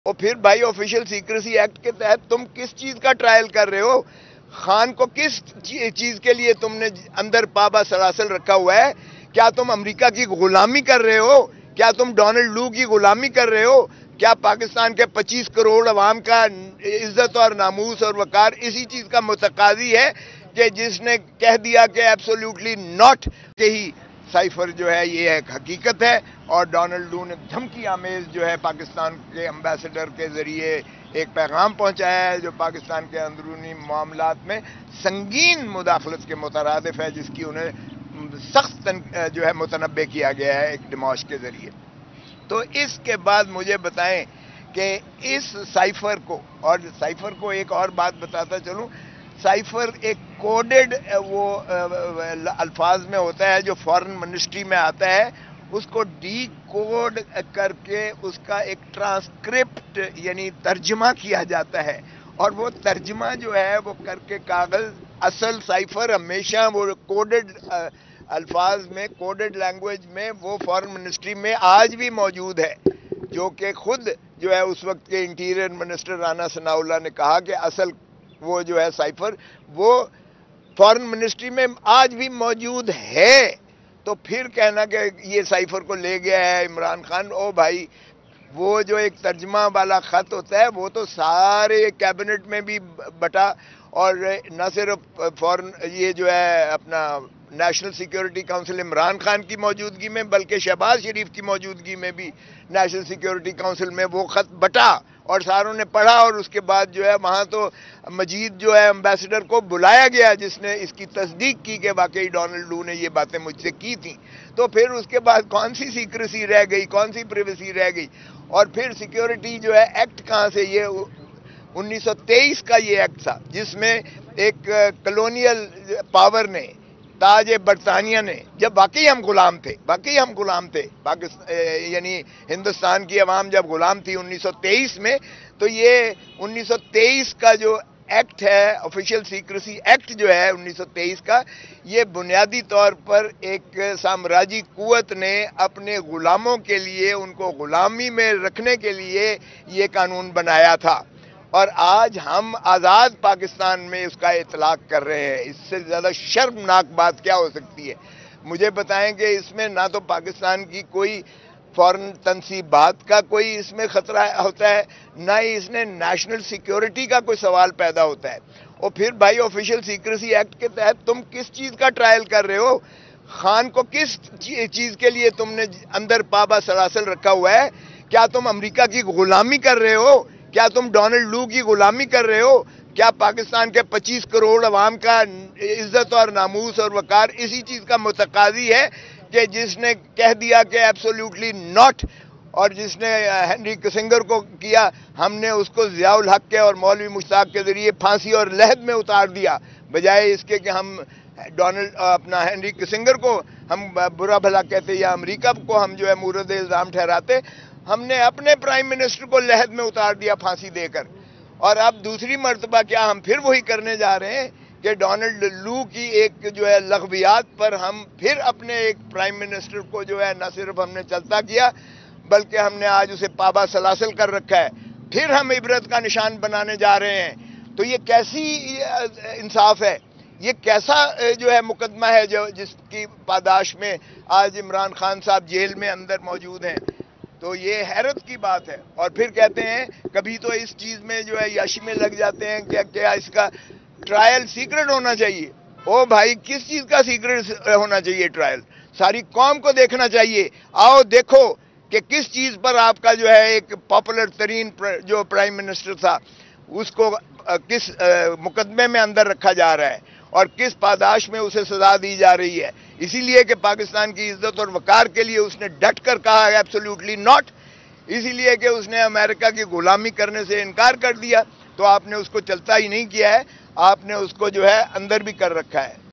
Latif Khosa's Big Statement Over Cipher Case Regarding Imran Khan | Capital TV
Latif Kohsa's Big Statement Over Cipher Case Regarding Imran Khan | Capital TV